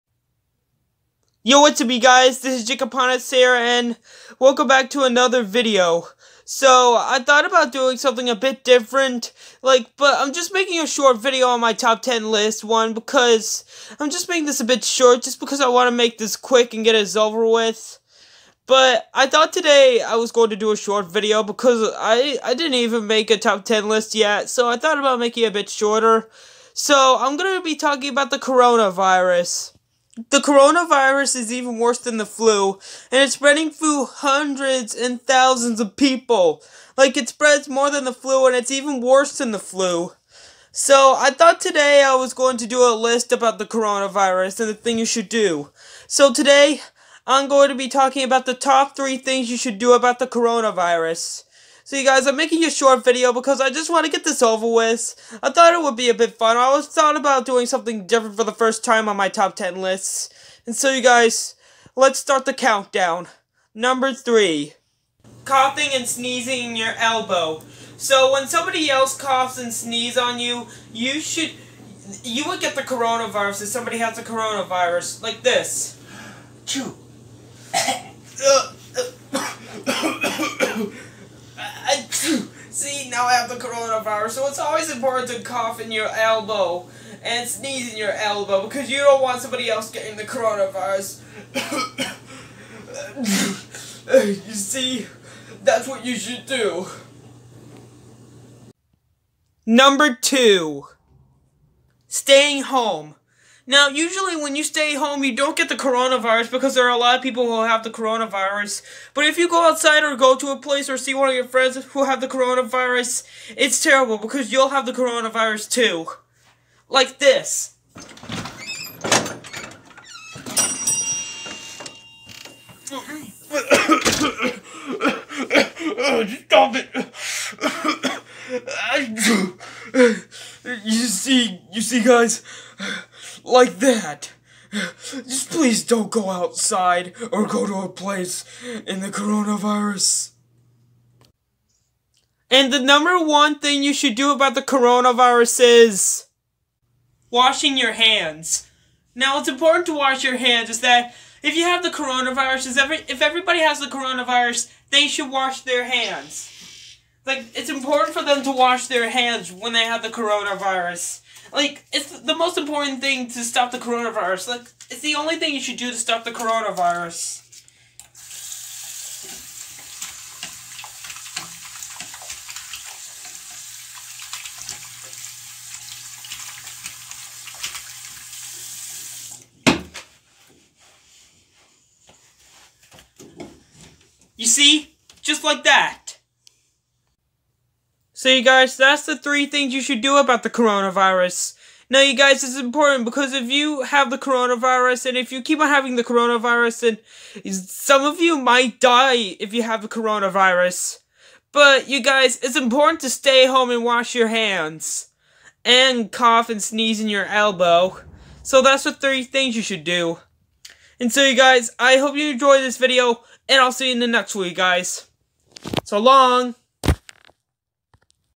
Top 3 Things You Should do About COVID-19
Jacob talks about things people should do about the coronavirus